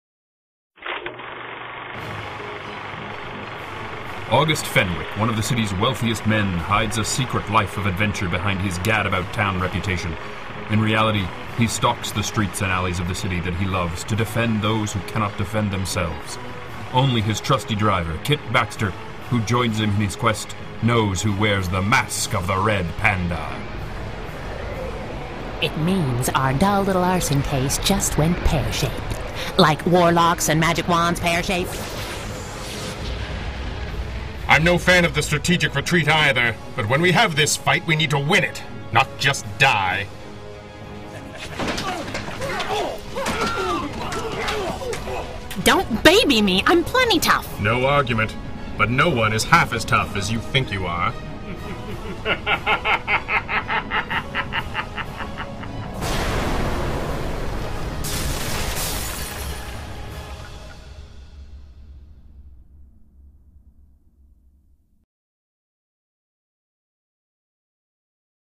Mask of the Red Panda trailer.mp4
Mask of the Red Panda - the terrific twosome of Toronto make their comic book debut Feb 27, 2013 with issue 1 of Mask of the Red Panda! 32 pages of pulse-pounding pulp perfection for just 99 cents! Written by Gregg Taylor, with art & letters by Dean Kotz. Available digitally from Monkeybrain Comics through Comixology